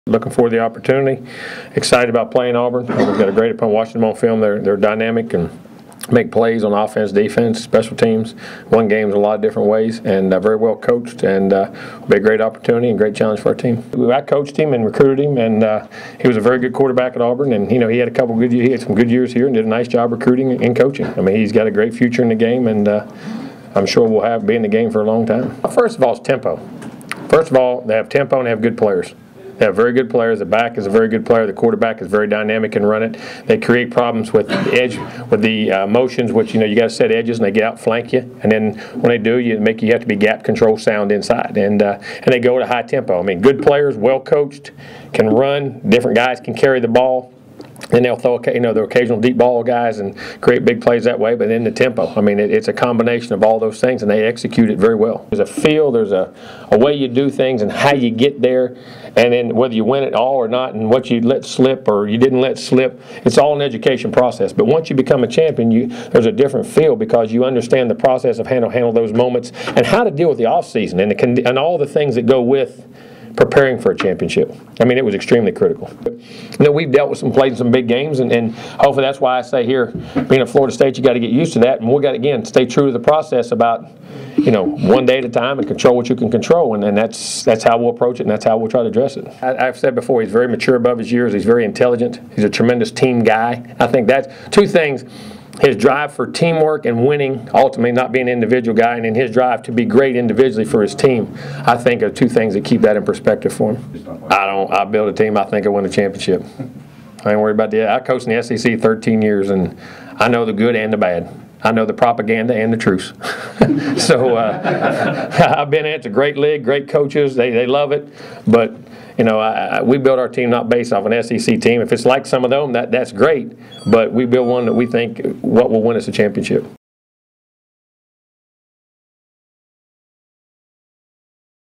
Press Conference Highlights: December 19
Courtesy Florida State head coach meets with the media on Thursday.  Watch the highlights here.